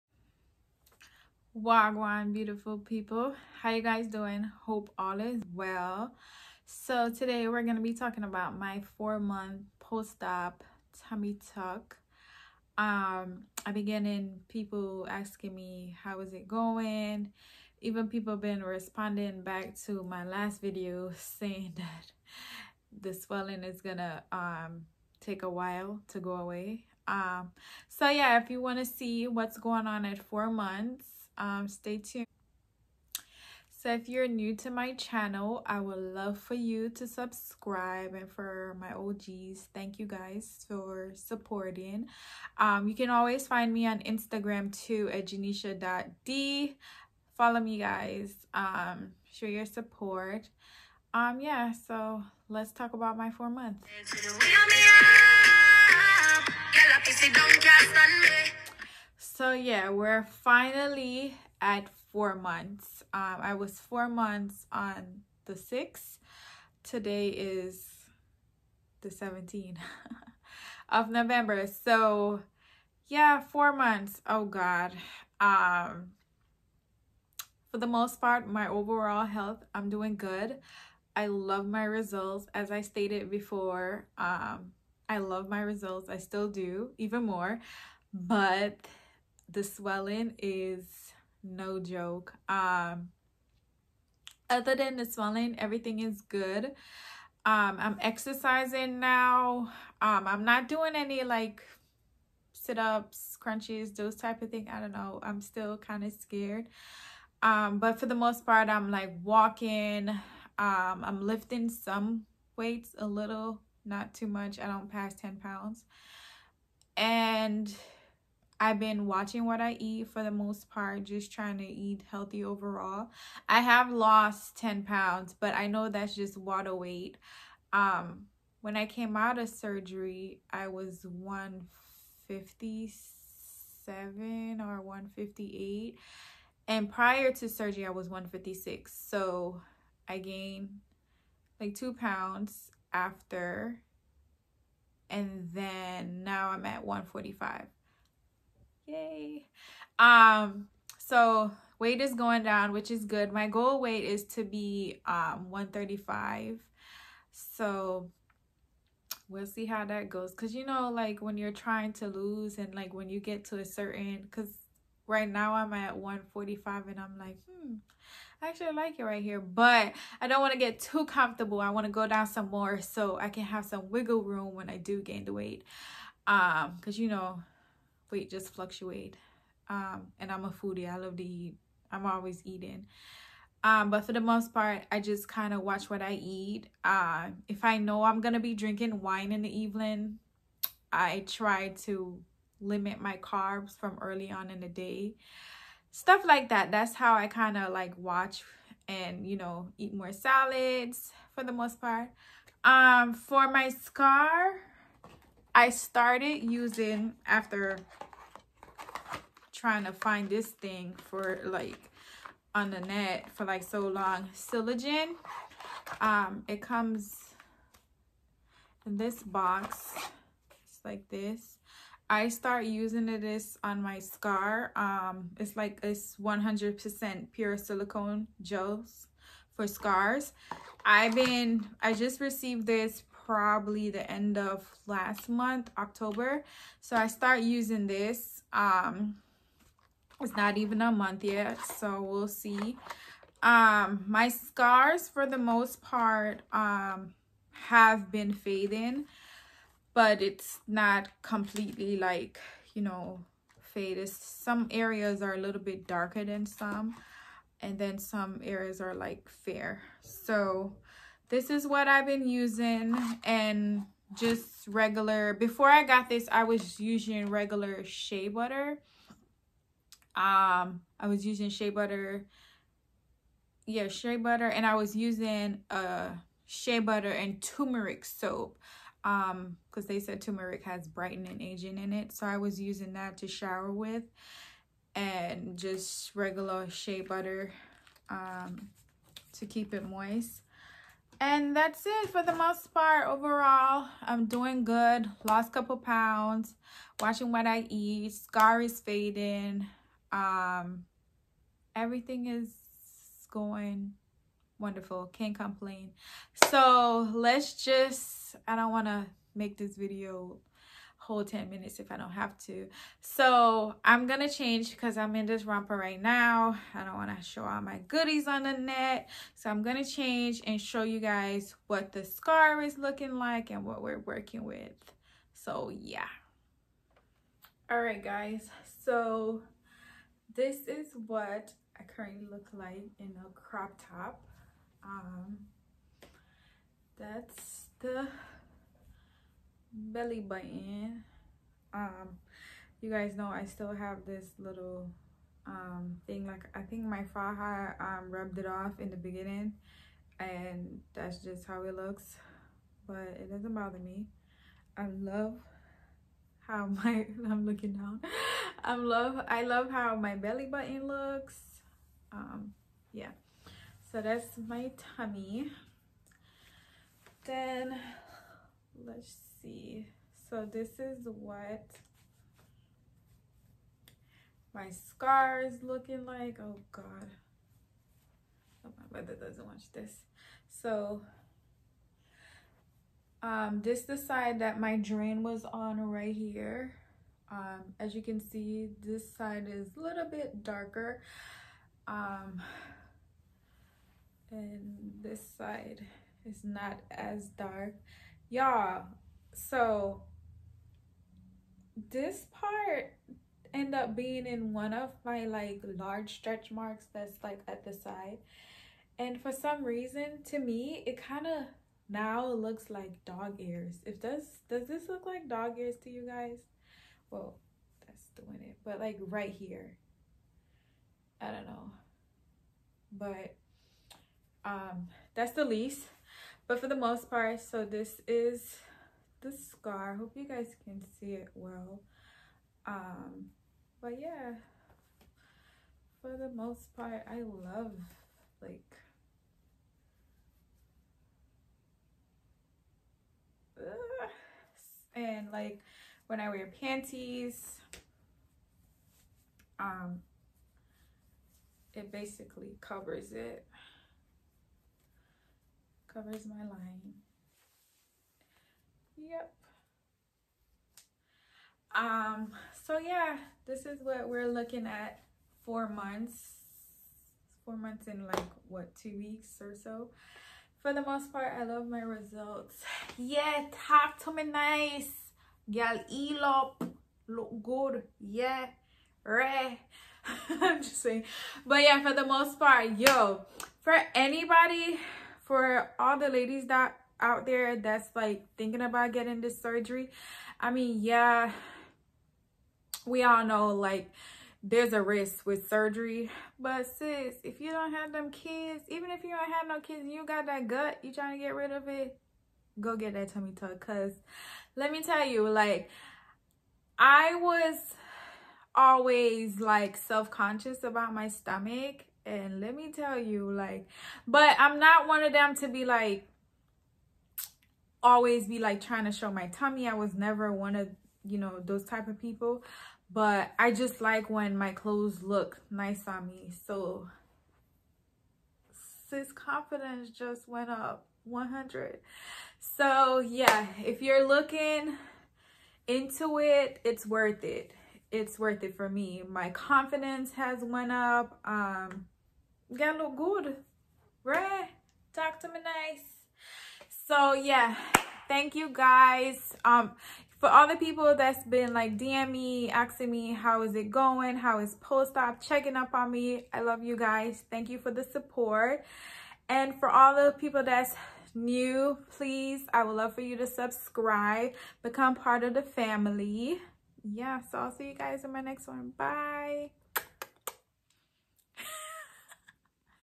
4 MONTHS POST TUMMY TUCK
Hey beauties today I will be showing and talking about my 4 months post op tummy tuck procedure that i had done in Santo Domingo DR. my doctor was Kelvin Eusebio. i had tummy tuck with 360 liposuction. My overall experience with the doctor and his staff was wonderful.

ROMANS 8:18 I consider that our present sufferings are not worth comparing with the glory that will be revealed in us.

Love Gen